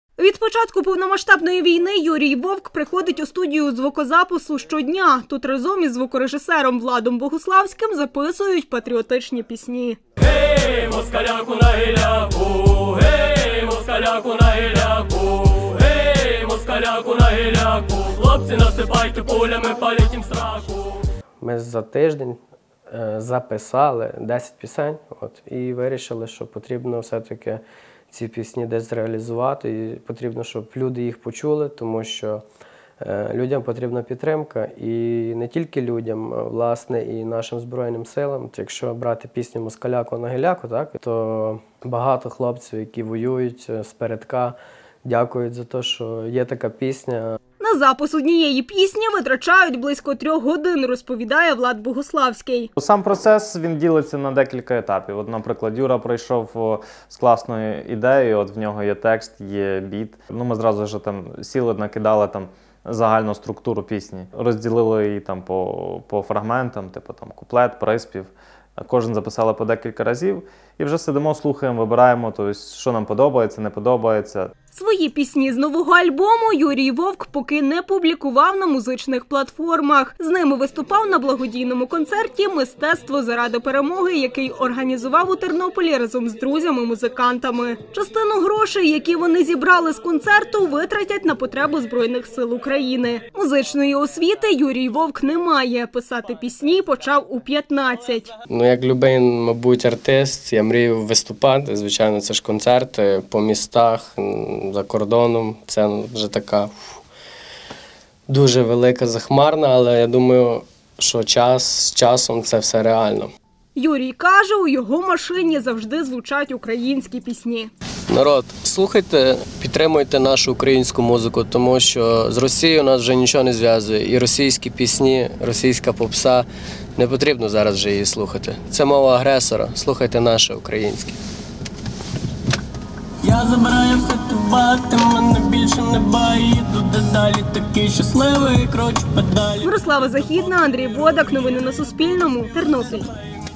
Тернопільський музикант записав альбом патріотичних пісень
23-річний музикант з Тернополя Юрій Вовк записав альбом патріотичних пісень. Хлопець каже, працював над ним від початку повномасштабної війни в Україні. У музичній збірці 10 пісень. Вони про Україну, українців та їхню долю.